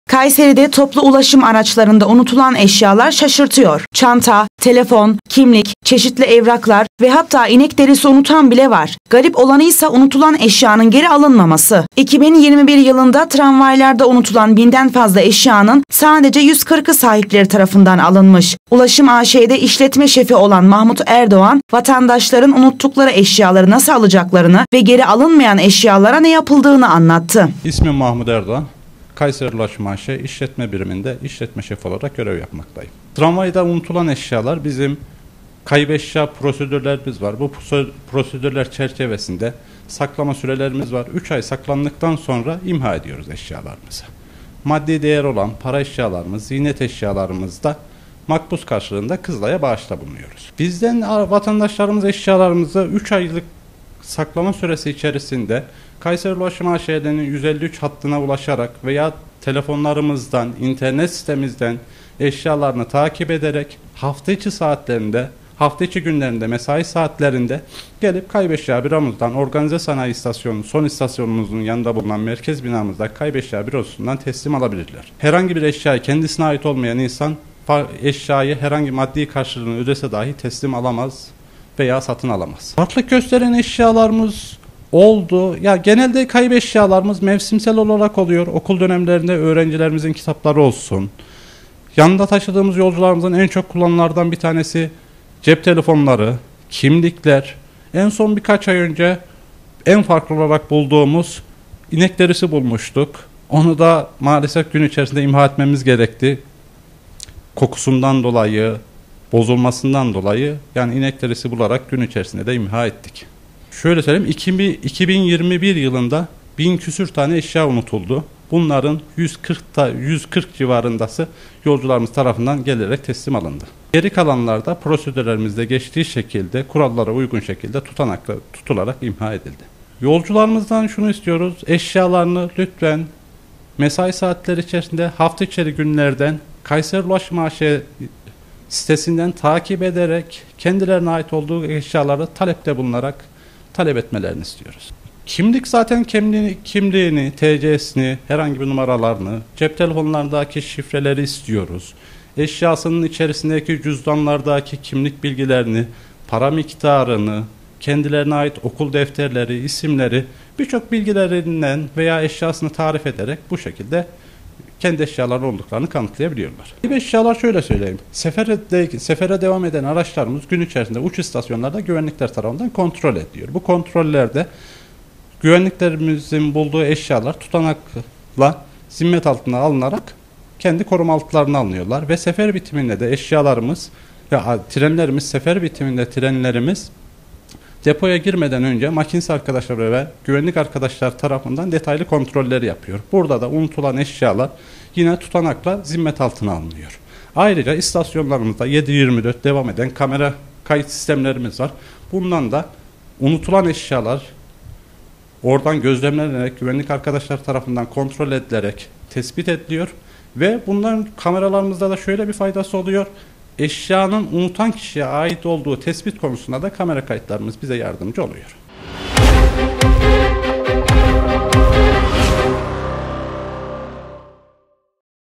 Toplu ulaşım araçlarında unutulan eşyaların çoğu geri alınmıyor
KAYSERİ, (BGC - ÖZEL HABER) – Kayseri’de toplu ulaşım araçlarında unutulan eşyalar şaşırtıyor. Çanta, telefona, kimlik, çeşitli evraklar ve hatta inek derisi unutan bile var. Garip olanı ise unutulan eşyanın geri alınmaması. 2021 yılında tramvaylarda unutulan binden fazla eşyanın sadece 140’ı sahipleri tarafından geri alınmış. Ulaşım A.Ş.'de İşletme Şefi olan Mahmut Erdoğan vatandaşların unuttukları eşyaları nasıl alacaklarını ve geri alınmayan eşyalara ne yapıldığını anlattı.

Kayseri'de toplu ulaşımı kullanan bazı vatandaşlar, yanlarında bulunan bazı eşyaları araçta unutup evine gidiyor. Nerede unuttuğunu da bilemeyen vatandaşlar, eşyasını geri almak için harekete de geçmiyor. Oysa ki toplu ulaşım araçlarında unutulan eşyalar, kayıt altına alınarak kayıp eşya bürosunda depolanıyor. Tramvay araçlarında 2021 senesinde binden fazla kişinin eşyasını unuttuğu açıklandı. Unutulanlar arasında, kimlik, çeşitli evraklar, şemsiye, bozuk para, çanta, telefon ve hatta inek derisi bulunuyor.

KAYIP EŞYA DEPOSUNDA TOPLANIYOR

Araçlarda unutulan malzemelerin kayıp eşya deposunda tutulduğunu anlatan Ulaşım A.Ş. İşletme Şefi Mahmut Erdoğan, “Sefere çıkan araçlarımız gün içerisinde güvenlik personeli  tarafından kontrol ediliyor. Bu kontrollerde bulunan eşyalar tutanak tutularak zimmet altına alınıyor. Daha sonra da kayıp eşya büromuza teslim ediliyor.  Her tramvay durağında bulunan kamera sistemi sayesinde unutulan eşyaların da kime ait olduğunu öğrenebiliyoruz" ifadelerini kullandı.

"KAYIP EŞYALARI SADECE SAHİPLERİ ALABİLİR"

Tramvaylarda bulunan kayıp eşyaları sadece eşya sahiplerinin alabileceğini belirten Erdoğan, "Eşyalarını kaybeden vatandaşlar 3 ay içerisinde Kayseri Ulaşım A.Ş.'nin 153 numaralı hattına ulaşarak veya telefonlarımızdan internet sitemizden eşyalarını takip ederek hafta içi mesai saatlerinde merkez binamızda bulunan kayıp eşya büromuzdan alabilirler. Kayıp olarak bulunan eşyaları sahibi olmayan kişi para ödese bile o eşyayı satın alamaz" ifadelerini kullandı.

“BAZI EŞYALAR İMHA EDİLİYOR”

Araçlarda bırakılan eşyaların 3 ay süreyle sakladıklarını açıklayan Erdoğan, "Bizlerin kayıp eşya prosedürlerimiz var. Bu prosedürler kapsamında 3 ay bekletilen ürünler imha ediliyor. Maddi değeri olan para ve ziynet eşyaları ise makbuz karşılığında Kızılay'a bağışta bulunuyoruz" diye konuştu.

"TRAMVAYDA İNEK DERİSİ BULDUK"

Unutulan binlerce ürünün arasında çok farklı ürünler ile karşılaştıklarını belirten Erdoğan, "Tramvaylarda vatandaşlar kimlik ve telefonlarını çok fazla unutuyorlar. En farklı olarak yaklaşık 3 ay önce inek derisi bulduk. Onu da maalesef kokusundan dolayı gün içerisinde imha etmek zorunda kaldık" diye konuştu.